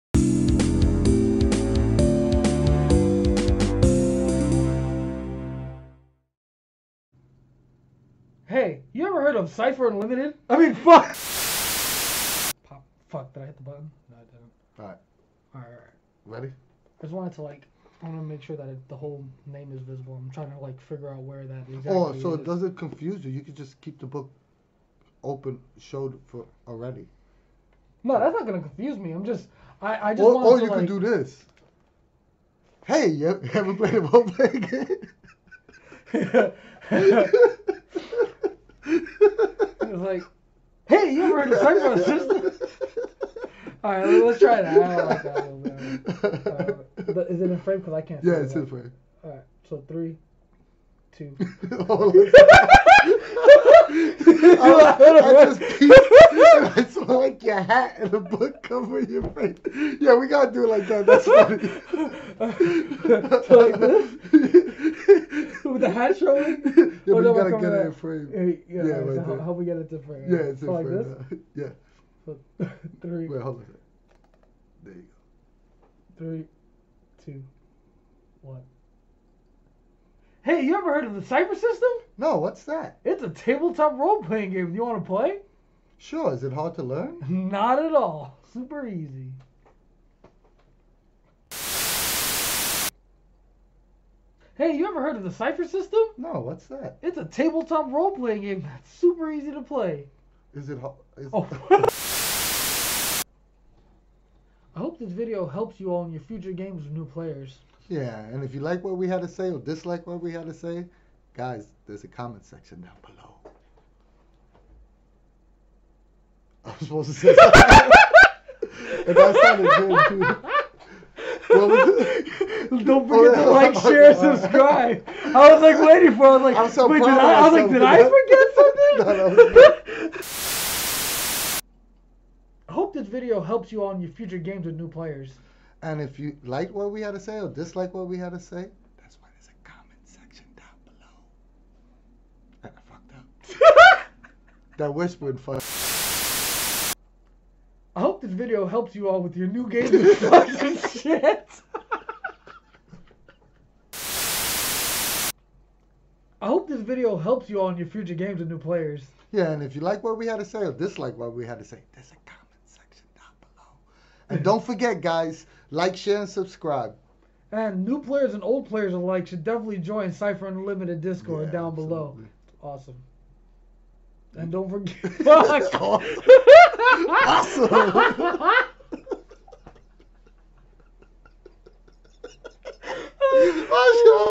New Player video outtakes
silliness from our messed up attempts at making the Introducing New Players video!